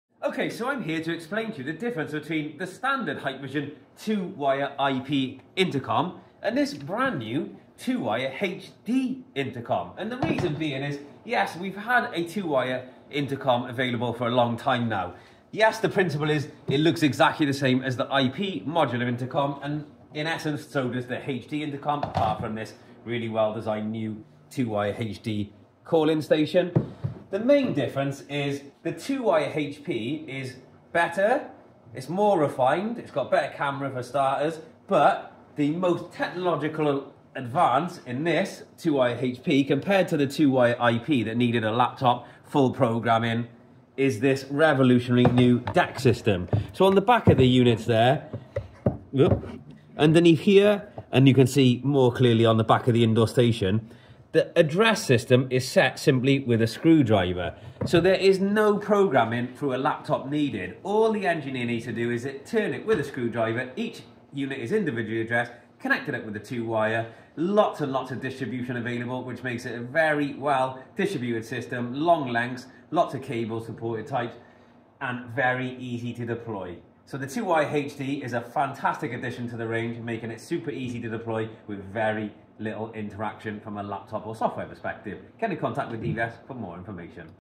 Hikvision 2-Wire IP vs 2-Wire HD — Which is Right for You? 🤔
🔹 Choose 2-Wire IP Products when:

You want smart integration with other IP systems.
Remote access and app control (via Hik-Connect) are important.
You’re working on larger or new installations where future expansion is likely.

🔹 Choose 2-Wire HD Products when:

You need a simple, reliable setup with clear video quality.
It’s a small-scale or retrofit project where existing wiring can be reused.
You want a cost-effective and quick installation solution.

Both systems use the same 2-wire simplicity — the right choice depends on your project needs! ⚡